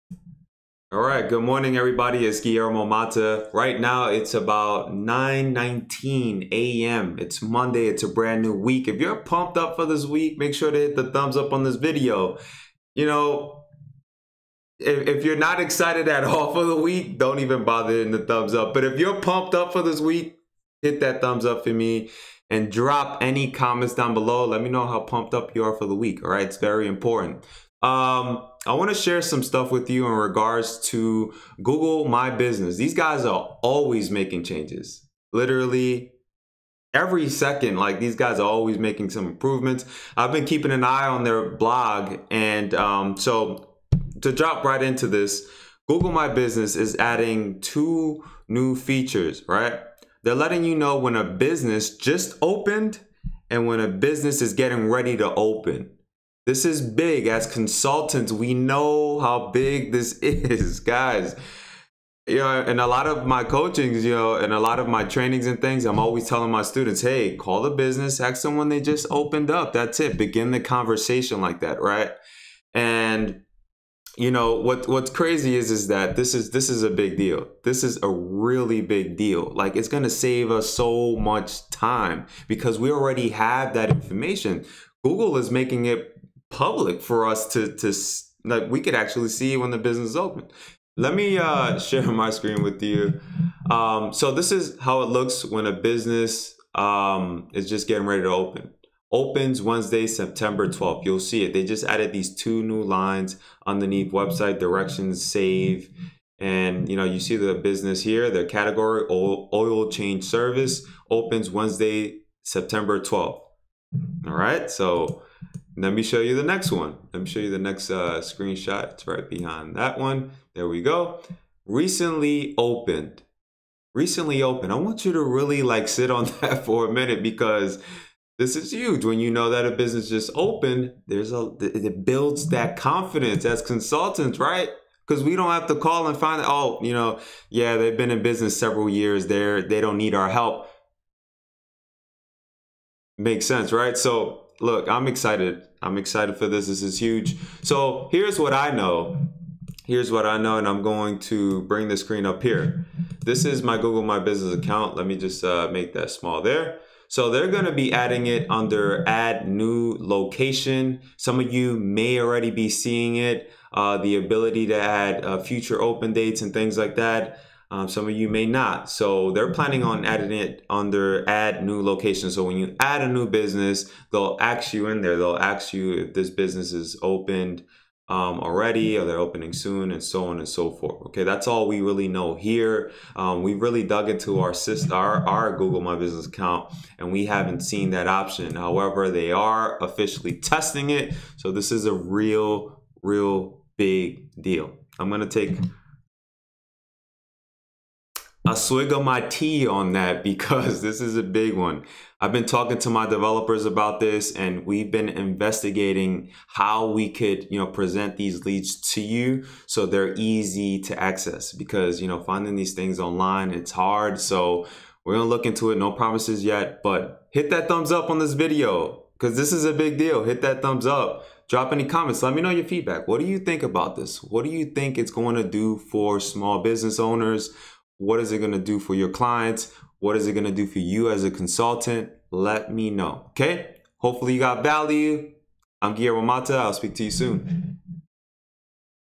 VLOG: GMB is testing future open dates for local businesses
Just found out GMB (Google My Business) is testing future opening
Dates and just opened for NEW local businesses!

(This is MAJOR)

Here’s what I cover in this video log:

I’ll show you where GMB plans on adding this feature.
We’ll go over where Google will be placing these notifications.
How this impacts local marketers.

MOBILE UPDATES:📱🔥
TEXT RTM to 67076 to be added to our
SMS list for mobile updates.